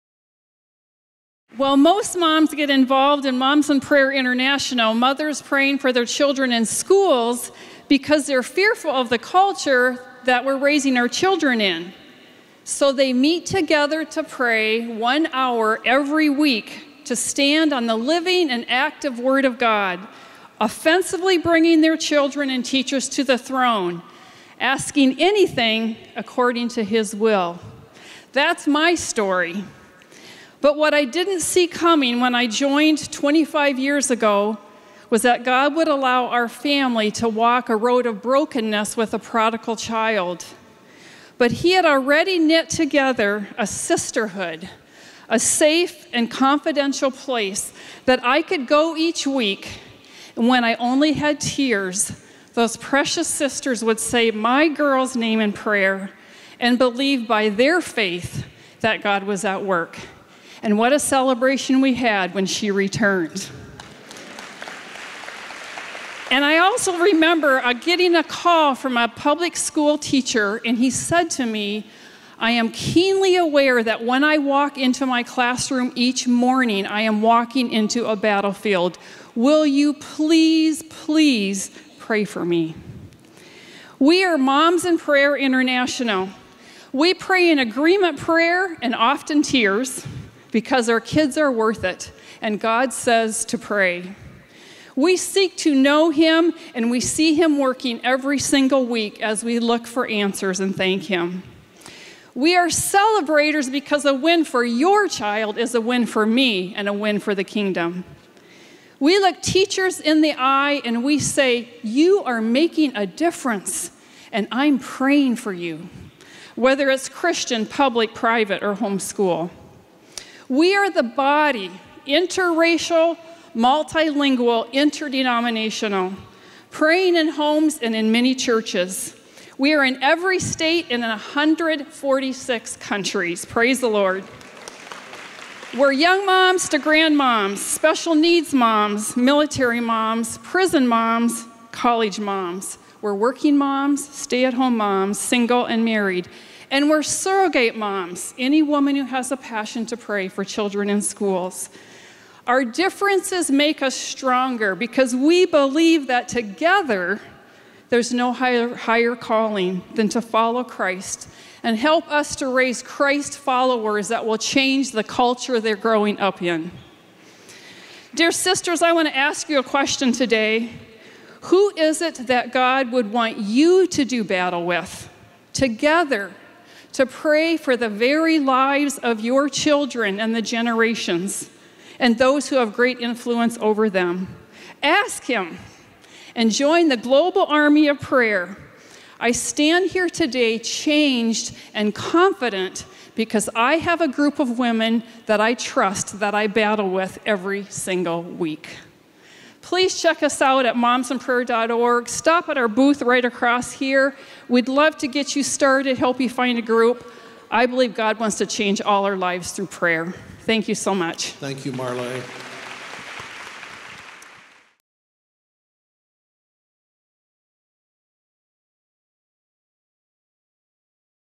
True Woman '16: Marlae Gritter—Moms in Prayer International
Marlae Gritter is the director of global advancement for Moms in Prayer International, a ministry that supports and equips moms to pray for their children and their schools. Watch to see Marlae’s passion for praying moms—and the impact they can have!

Videos like this are only possible with the support of viewers like you. Please consider giving generously to the work of helping women thrive in Christ